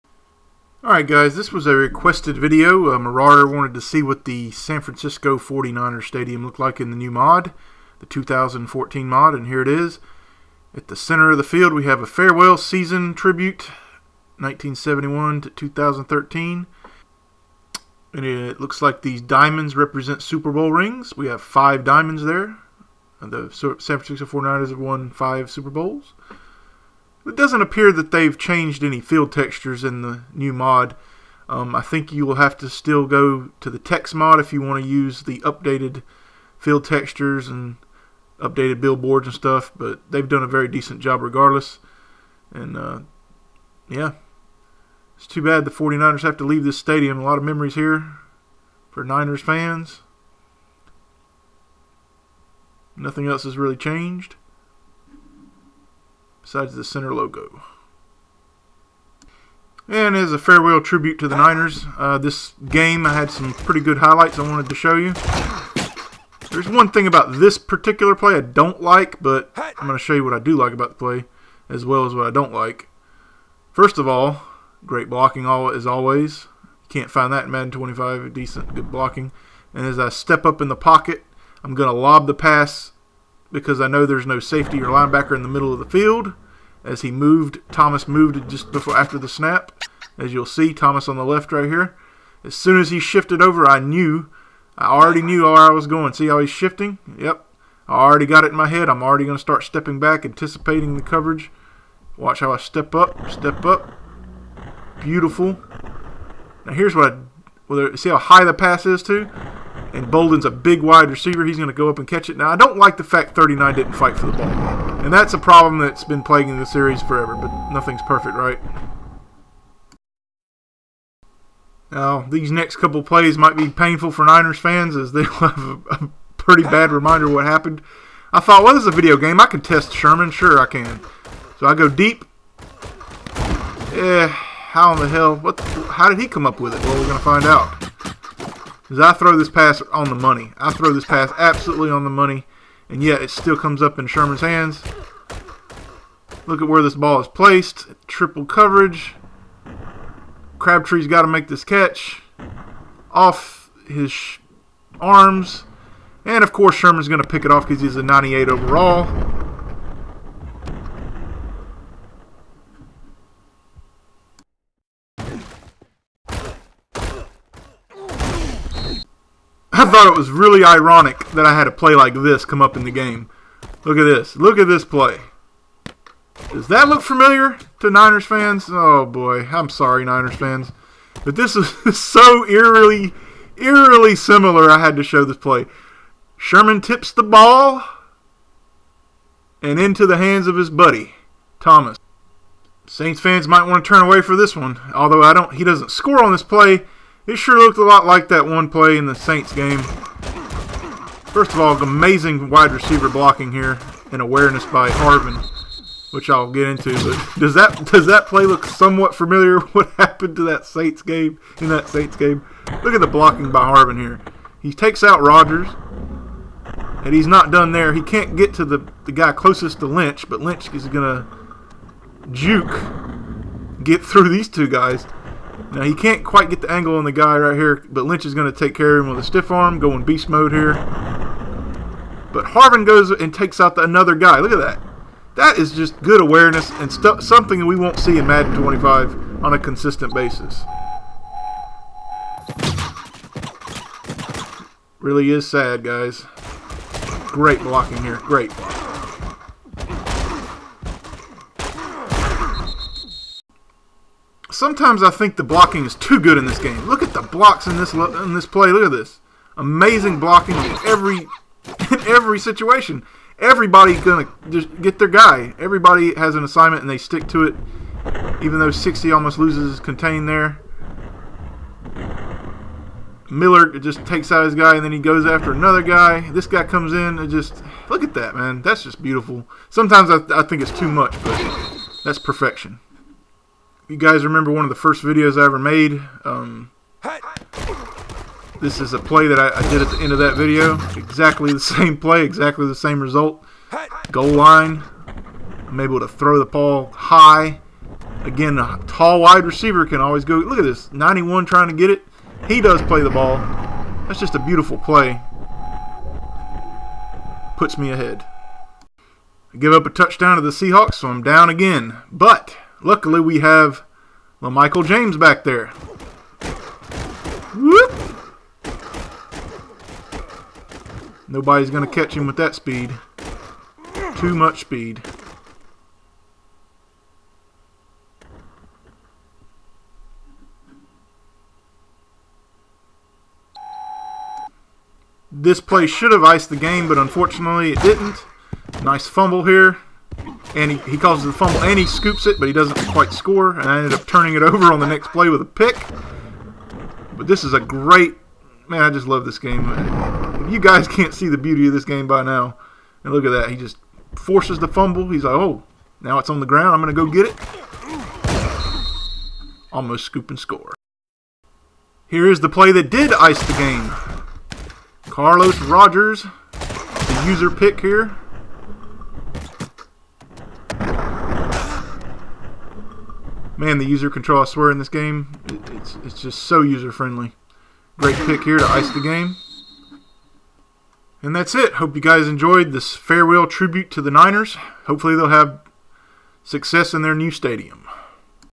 Farewell tribute to the Niners Stadium
This was a requested video by Marauder, he wanted to see what the Niners Stadium looked like in the new 2014 mod. I also show some  highlights and break down some plays in this video.